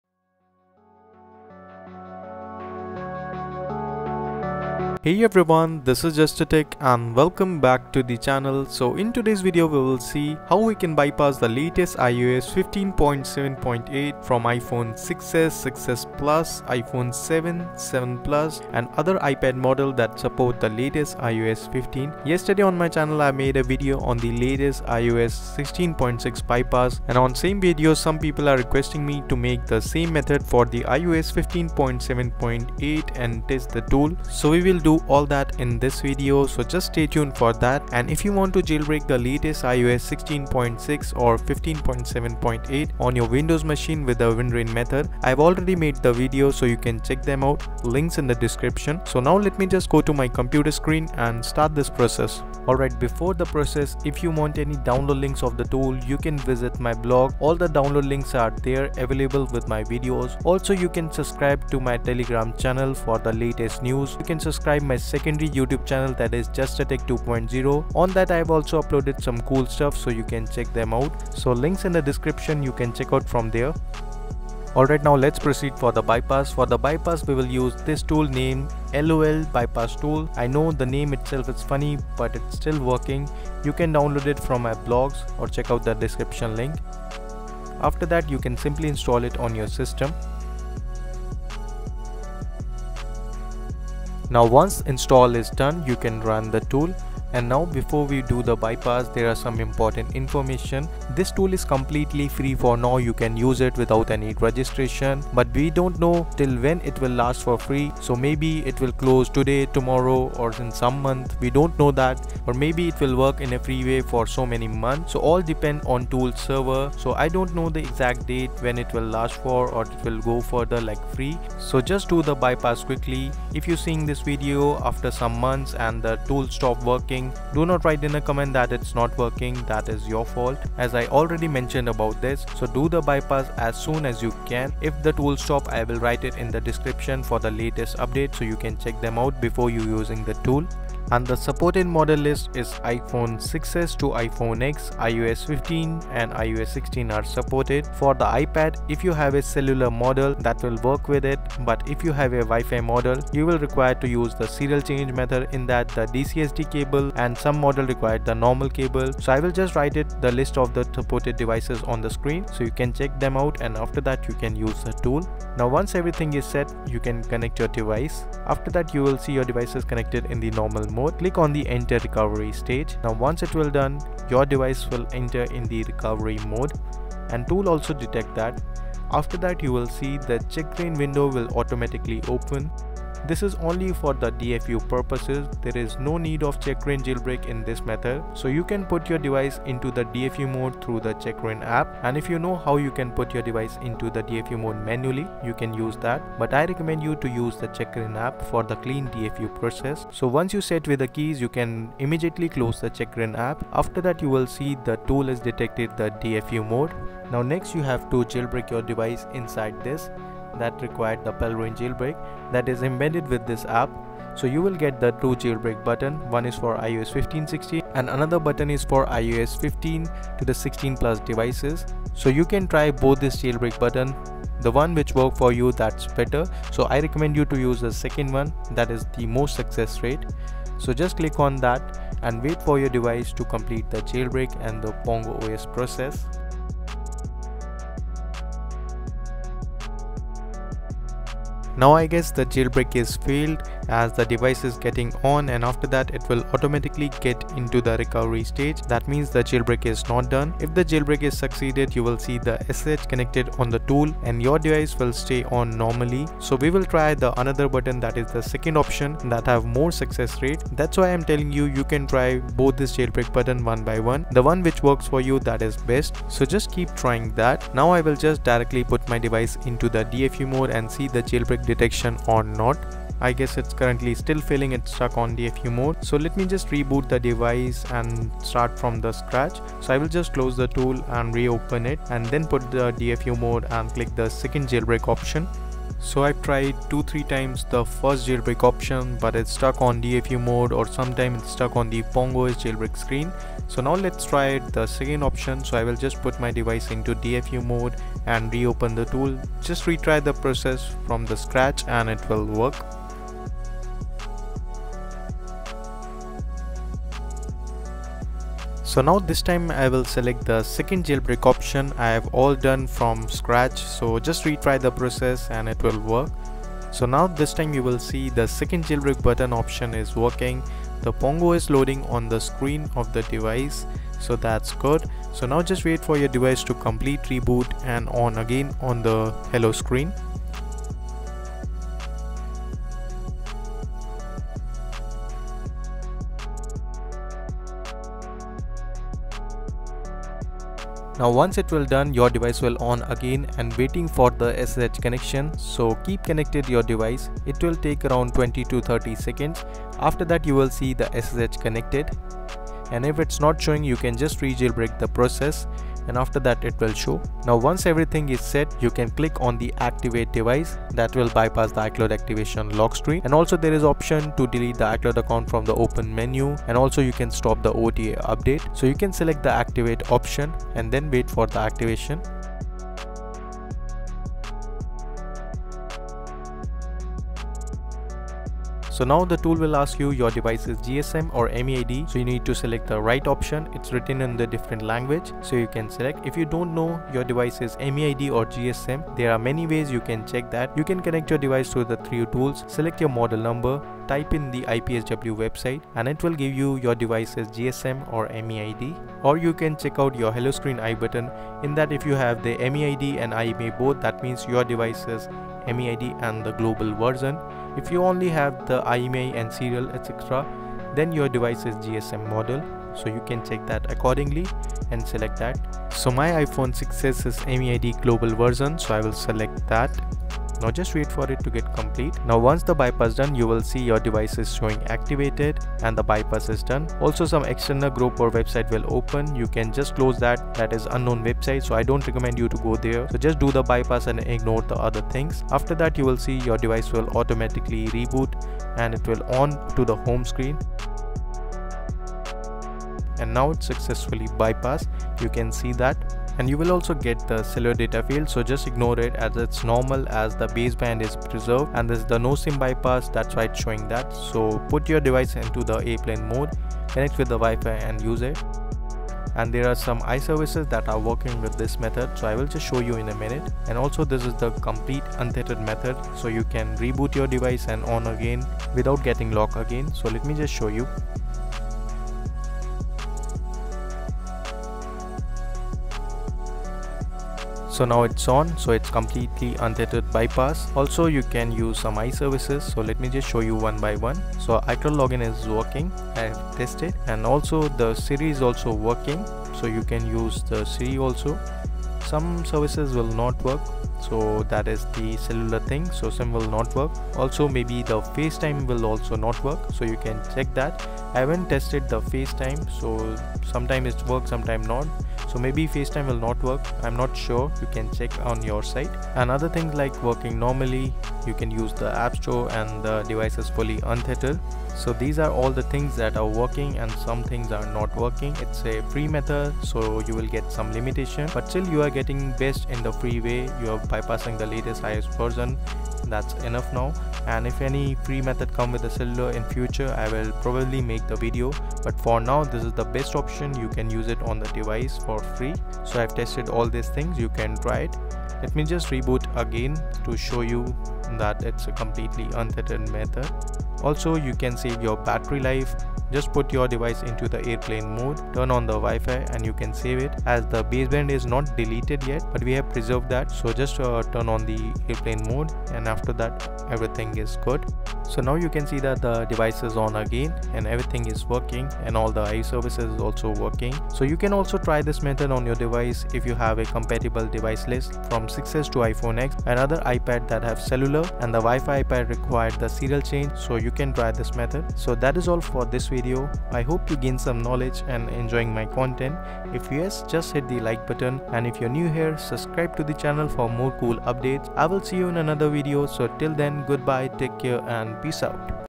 🔥✅ Free Untethered iCloud Bypass iOS 15.7.9/16.6 on iPhone/iPad| Checkra1n/PaleRa1n Jailbreak iOS 15
(Ad) Try AnyUnlock to Unlock Screen, Apple ID, Activation Lock, MDM & More

By Justatech
By iJustatech
By iamjustatech